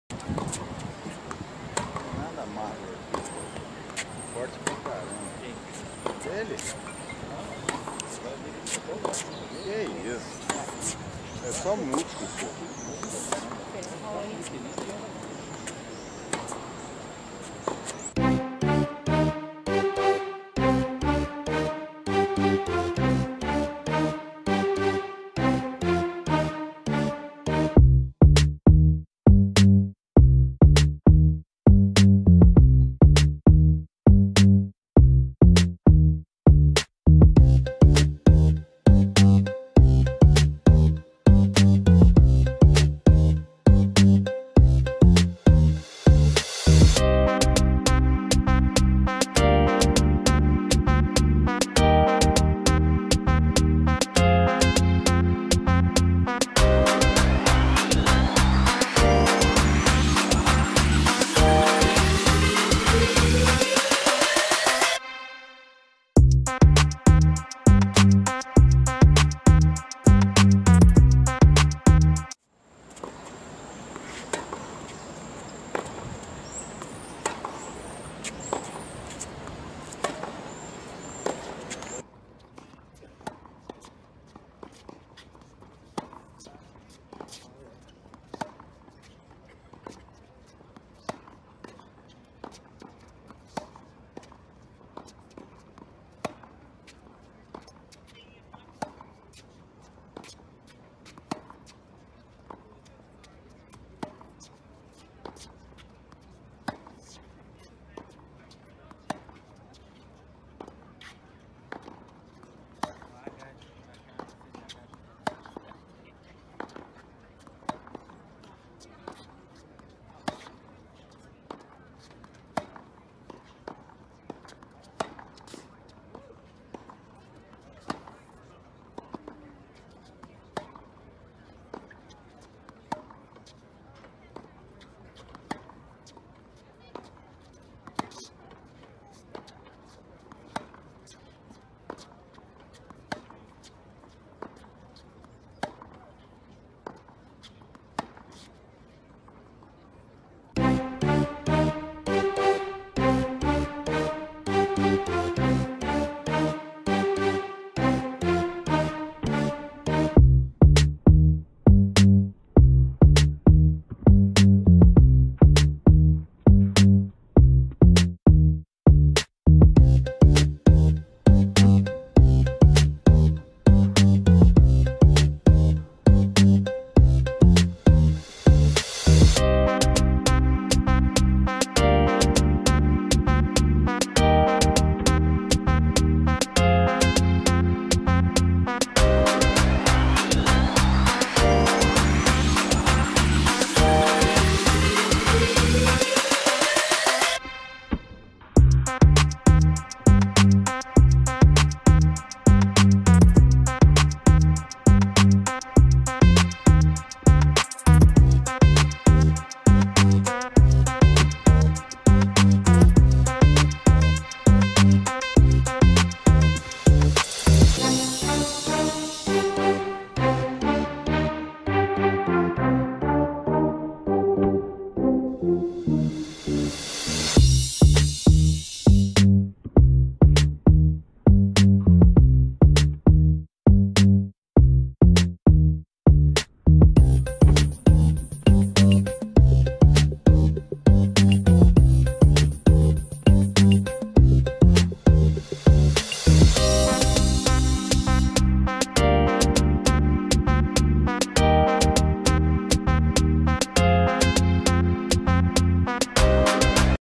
Roger Federer 페더러 포핸드 볼의 궤적에 따른 헤드의 위치 관찰하기👀
TENNIS COLLECTION!